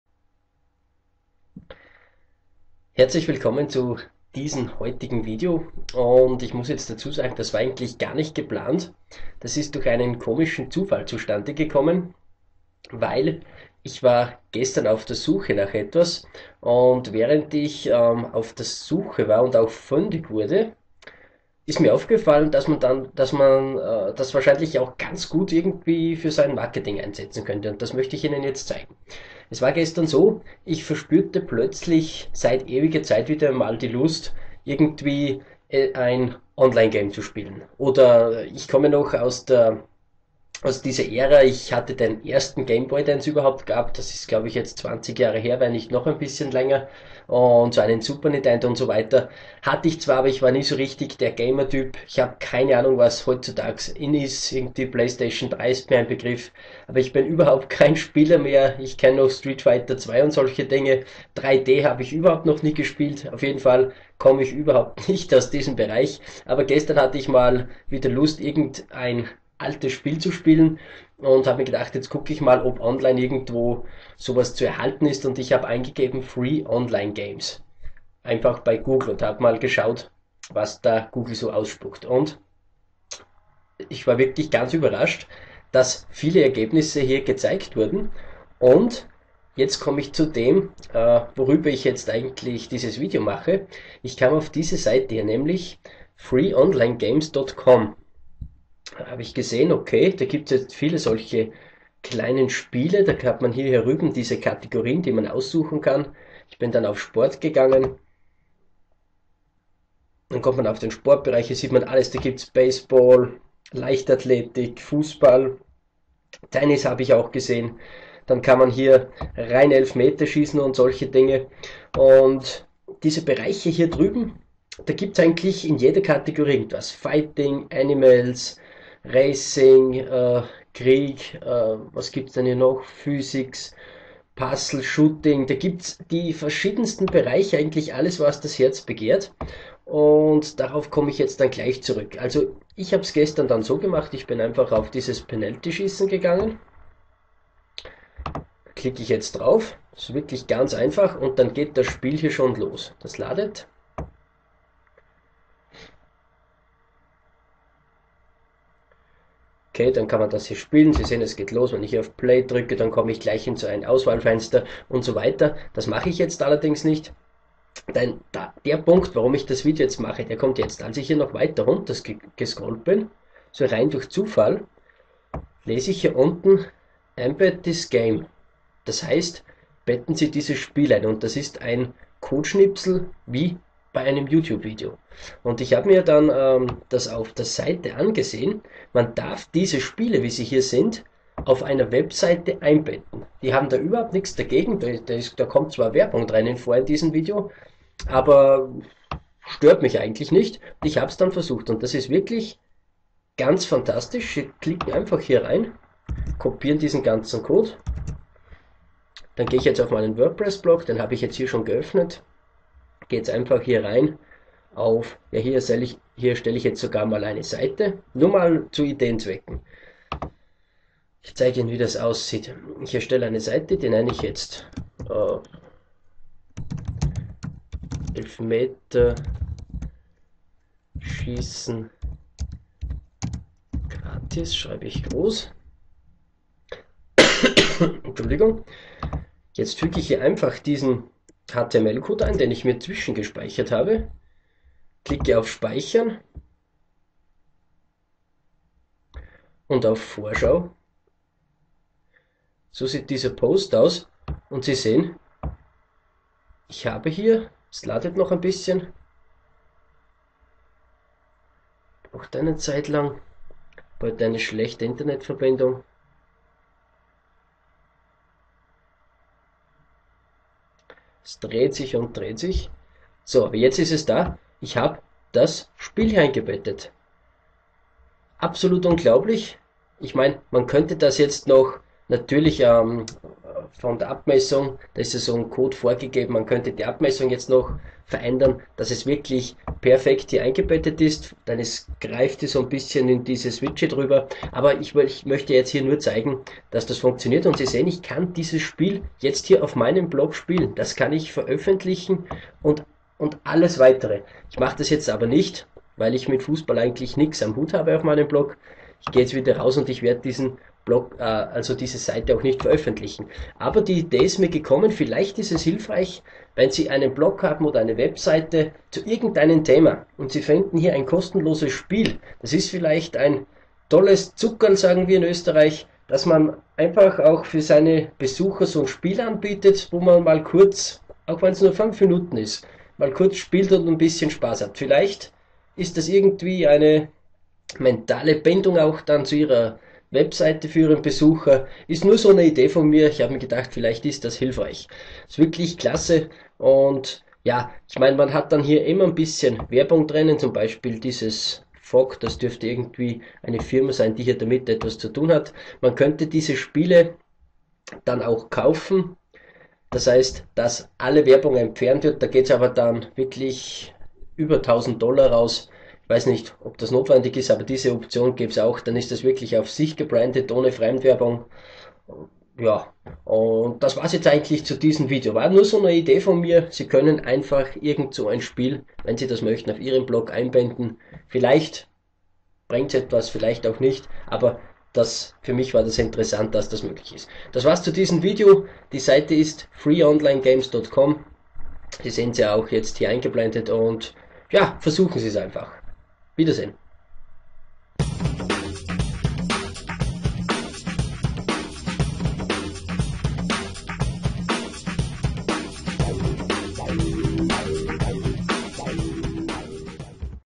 Free online Games für Ihre Webseite
Dieses Video ist durch einen Zufall zustande gekommen. Gestern verspürte ich seit langer langer Zeit wieder einmal die Lust ein Spiel zu spielen. Mit Spiel meine ich so wie früher der Game Boy oder das Super Nintendo war. Ich hatte vor ca. 20 Jahren auch mal einen Sega Megadrive. Muss aber dazu sagen, dass ich überhaupt kein sogenannter Gamer bin.

Mir ist zum Beispiel Playstation 3 ein Begriff, aber was man damit heute alles spielen kann, da habe ich überhaupt keine Ahnung. Auf jeden Fall begab ich mich bei Google auf die Suche und gab einfach Free online Games in die Suchleiste ein.

Viele Ergebnisse wurden ausgespuckt und irgendwie kam ich auf die Seite Freeonlinegames.com. Dort habe ich mich etwas umgesehen und wurde auch fündig.
Da gab es zu beinahe jedem Bereich eine große Auswahl an spielen. Eines habe ich dann auch gespielt und als ich dann noch etwas runter scrollte, entdeckte ich die Option „Embed Game on your Website". Das war genauso ein Code zum Einbetten, wie man diesen in Youtube Videos kennt, wenn man diese auf einer Webseite einbetten will.

Das habe ich dann auch versucht und es hat problemlos funktioniert. Ich konnte das Spiel sofort auf meinem eigenen Blog spielen.

Da kam mir die Idee zu diesem Video. Warum nicht einfach zu seinem Thema über das der jeweilige Blog oder die Webseite handelt ein kostenloses Video Game zum spielen anbieten.

Eventuell baut das noch mehr Vertrauen mit Ihrem Webseiten oder Blogbesucher auf als bisher.

Sehen Sie sich einfach auf der Seite ein wenig um.

Ich bin mir sicher, dass auch Sie zu Ihrem Thema ein tolles Spiel finden, dass Sie auf Ihrer Webseite kostenlos anbieten können.

Gruß
Benjamin